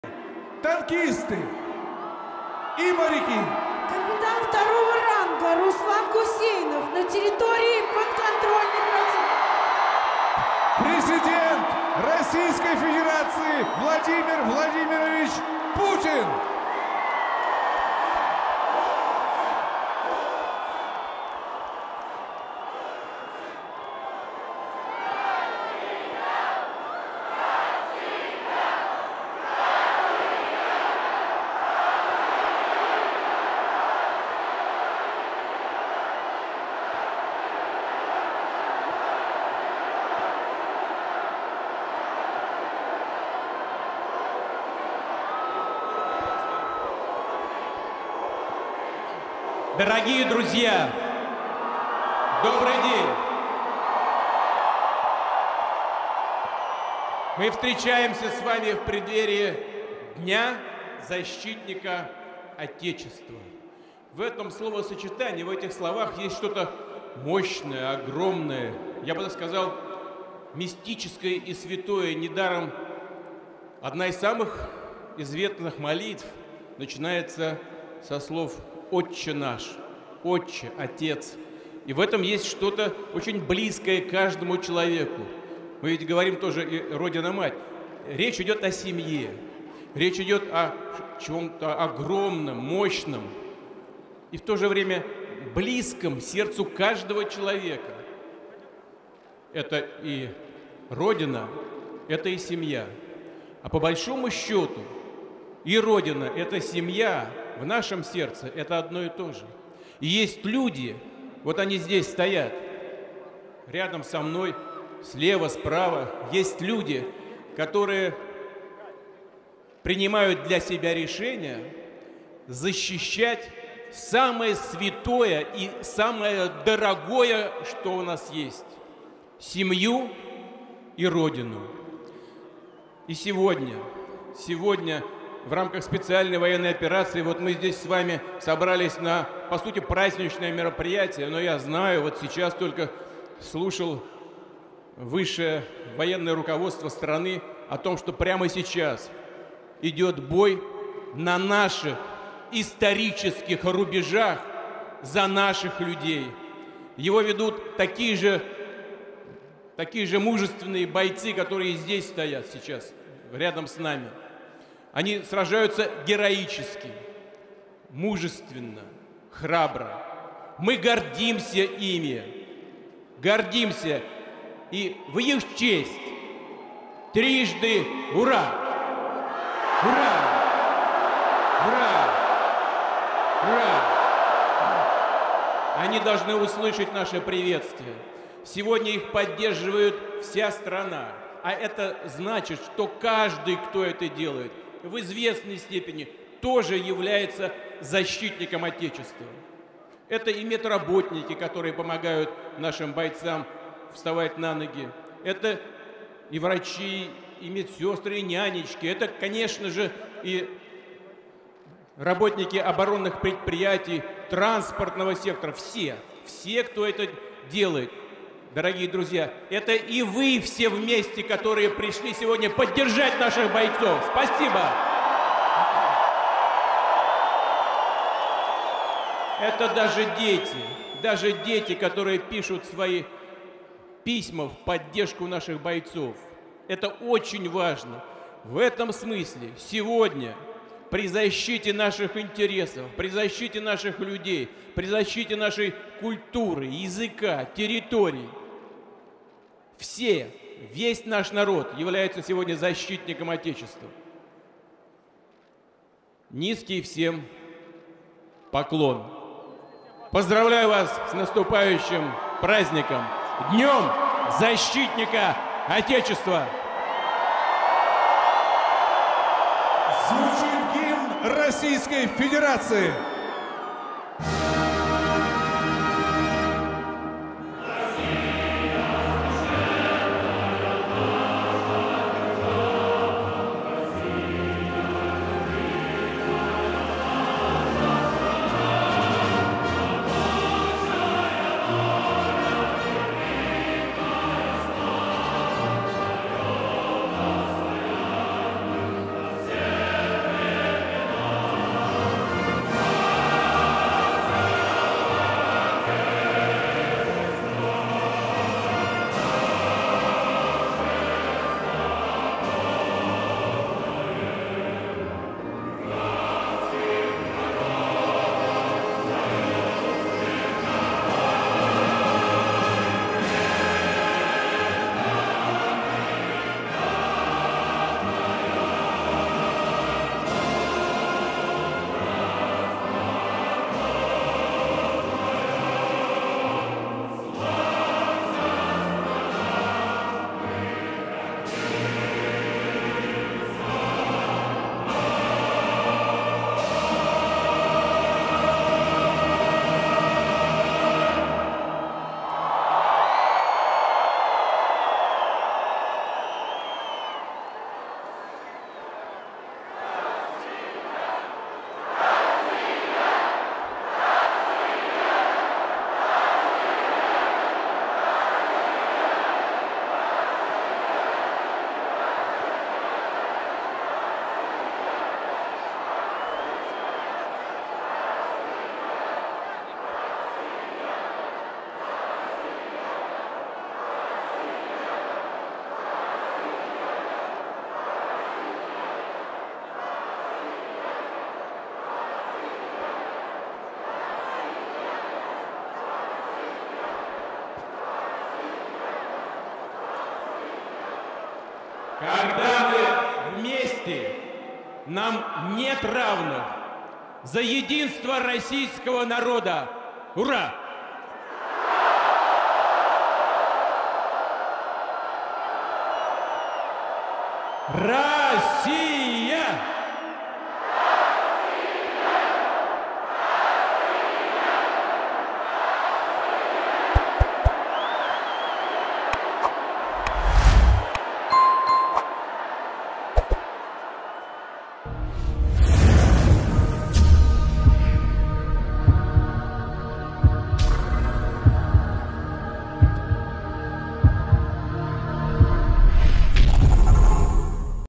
Vlagyimir Putyin beszéde a Luzsnyiki Stadionban (magyar felirattal)
Vlagyimir Putyin beszéde a Luzsnyiki Stadionban a Haza Védelmezőinek Napja előtt, a különleges hadművelet támogatására rendezett nagygyűlésen.